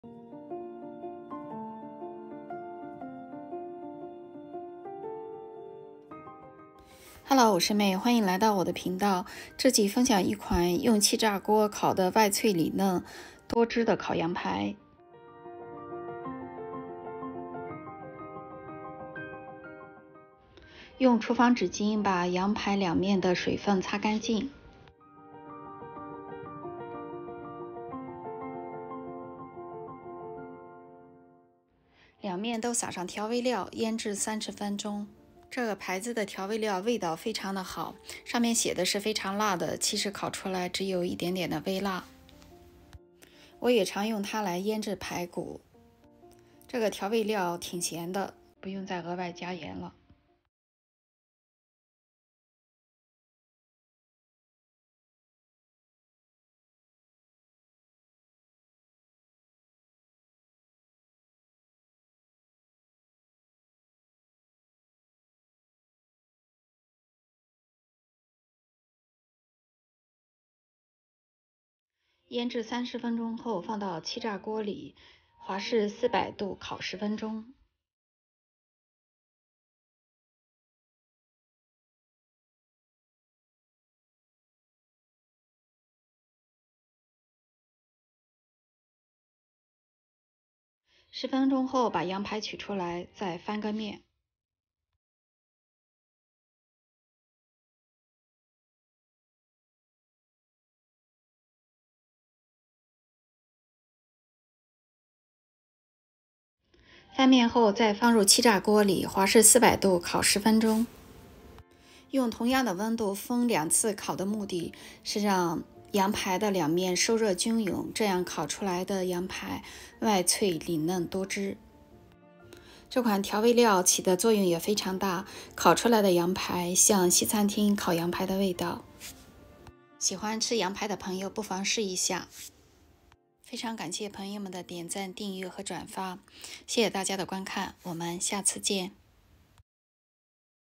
Air Fry Lamb Steak Recipe 气炸锅羊排
歡迎來到May梅日一品頻道！梅日一品，品味生活！
分享一个小技巧可以用气炸鍋出烤出外脆里嫩多汁的烤羊排。省時省力零失敗配方
食材
一块羊排
一些調味料 选用 Famous Dave’s devil’s spit seasoning
做法：兩面撒上調味料腌製30分鐘后，放入气炸鍋华氏400度摄氏204度烤10分鐘后，取出翻面再用同樣的溫度烤10分鐘就可以了

This is air fryer lamb Steak recipe. It’s crispy outside and juicy inside, and very easy to make it at home.

Ingredients
A piece of lamb chop
Some Famous Dave’s devil’s spit seasoning
Cook method:
Sprinkle the seasoning on both sides of the lamb, and waiting for 30 mins. Fry both sides for 10 minutes each at 400 degrees F